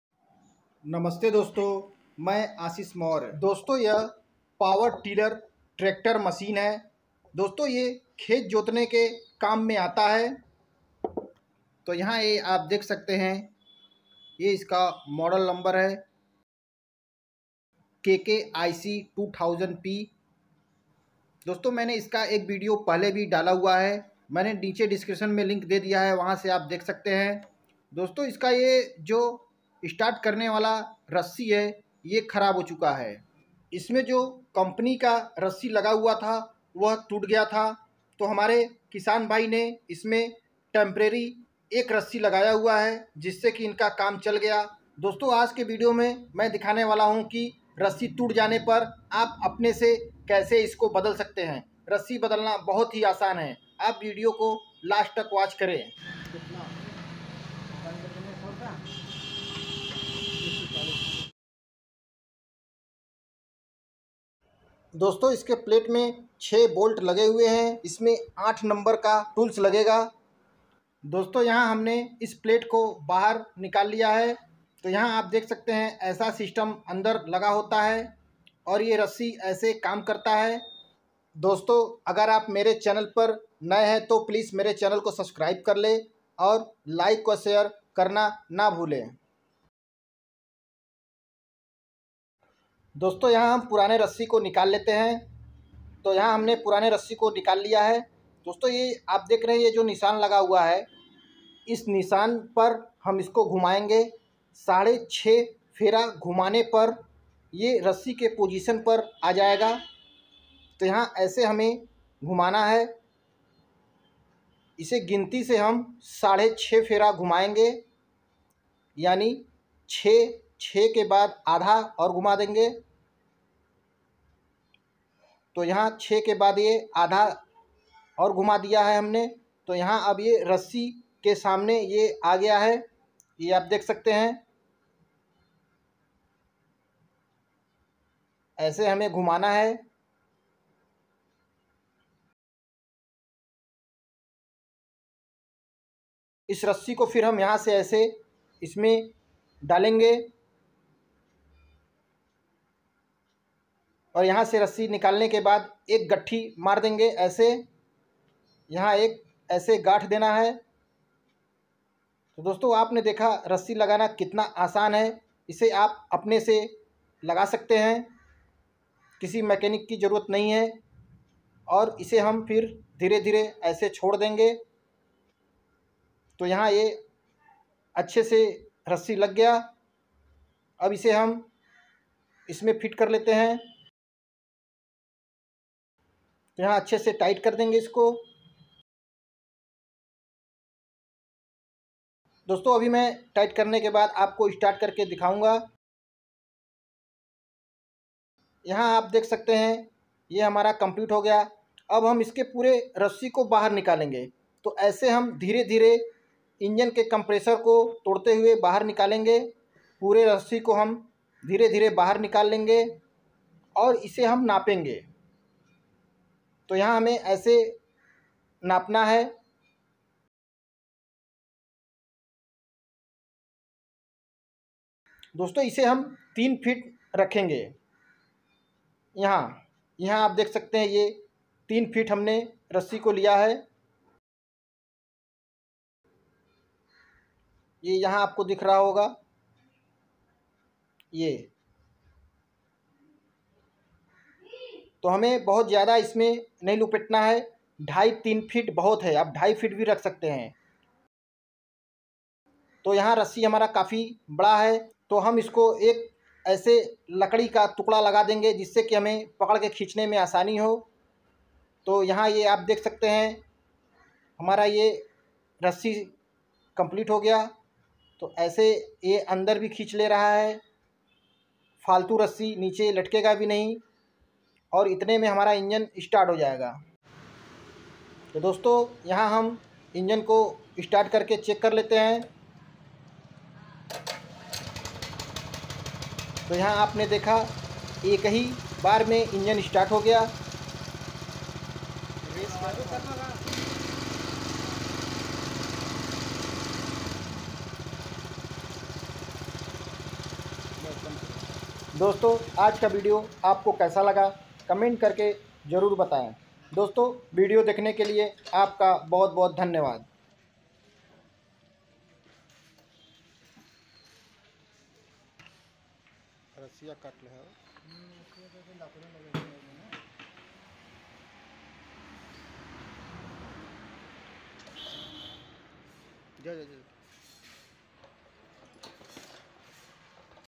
Power Tiller Machine || स्टार्टिंग रस्सी टूट जाने पर अपने से कैसे बदले
नमस्ते 🙏दोस्तो !!

आपका मेरे चैनल पर स्वागत है 🤗!!

दोस्तों इस वीडियो में मैन दिखाया है कि Power Tiller Machine का स्टार्टिंग रस्सी टूट जाने पर अपने से कैसे बदले।

Queries solved:-
1.Power Tiller Machine का स्टार्टिंग रस्सी टूट जाने पर अपने से कैसे बदले

Hello🙏friends !!

Friends , In this video I have showed how to fix broken starting rope of power tiller machine.

Queries solved: -
1.How to fix broken starting rope of power tiller machine.